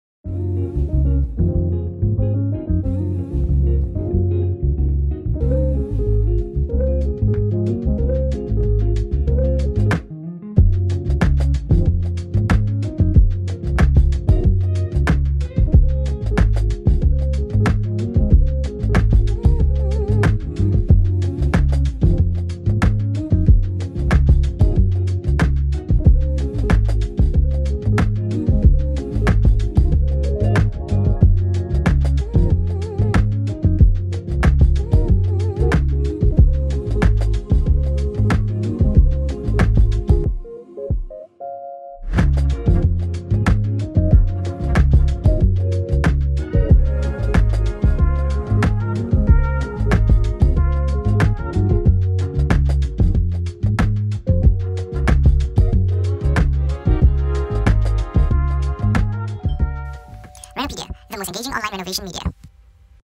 Creating a Modern Yet Warm Home | Matrix Interior
The warm colors of the interior are perceived as pleasant. Orange and yellow are often used when you want to express or cause feelings. The active effect makes it perfect for your home!

Designed by: Matrix Interior

and get free interior design plans from various interior designers and save your trouble from visiting dozen of interior firms.